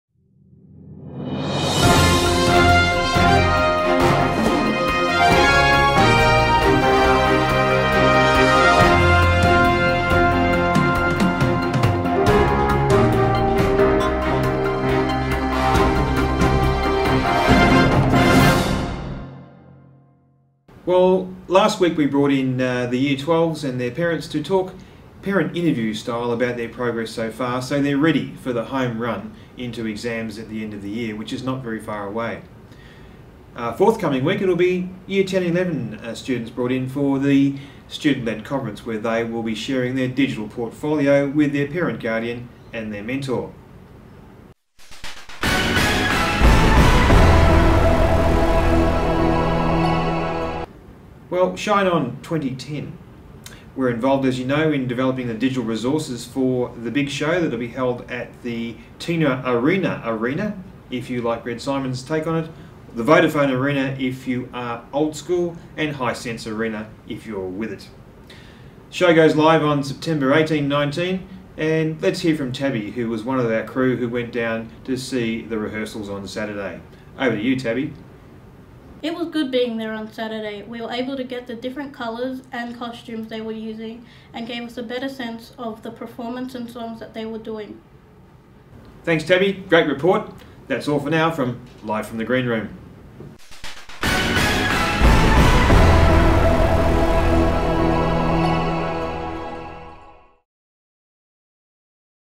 Episode 4: Shine On - Live From The Green Room
In this episode of Live From The Green Room, we look at the latest news on Coburg Senior High Schools involvement in Shine On, Victorias state school spectacular.